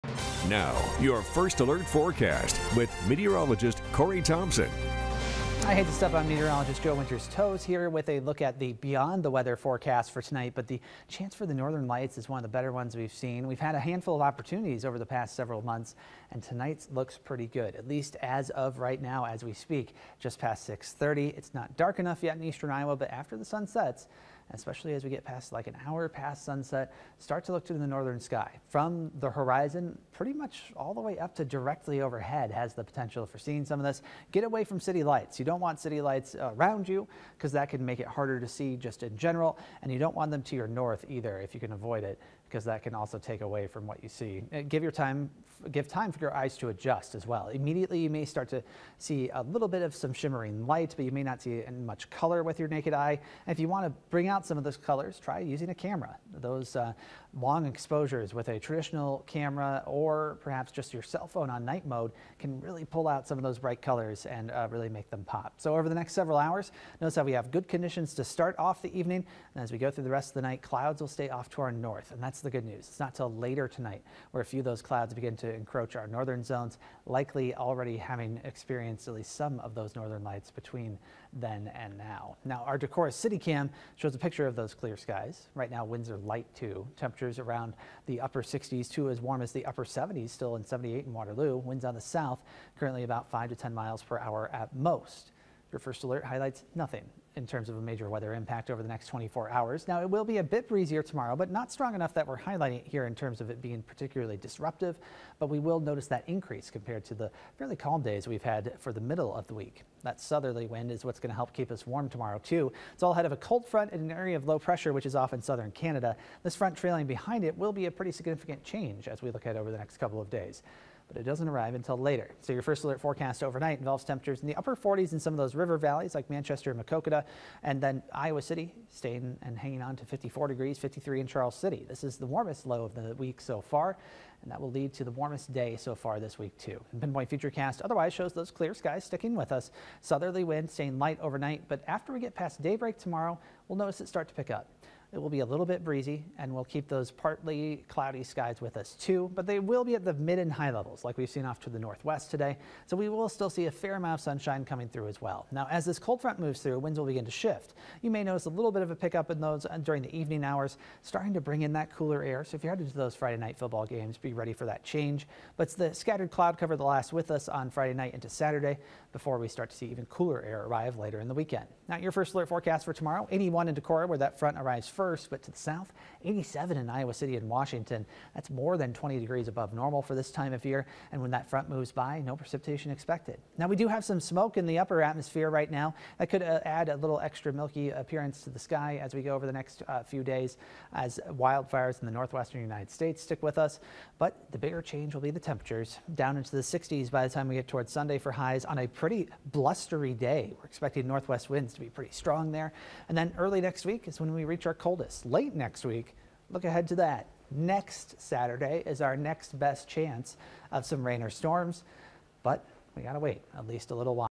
First Alert: Weather Now - First Alert Forecast for October 10
First Alert Meteorologist Corey Thompson gives some tips for viewing the Northern Lights in eastern Iowa on Thursday night.